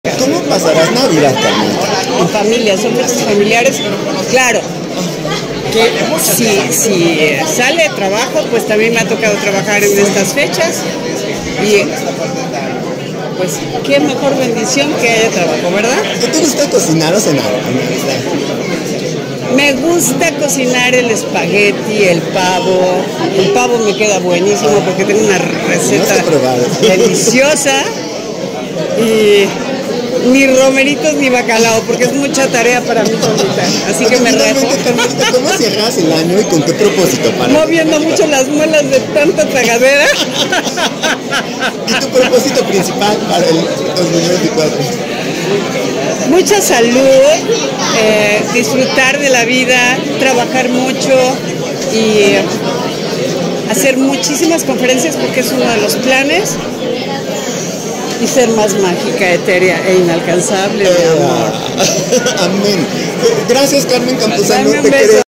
Esto cocinará en Navidad Carmen Campuzano y así despedirá el Año Viejo.
Carmen Campuzano nos platica qué cocinará esta Navidad y cómo despedirá el Año Viejo 2023.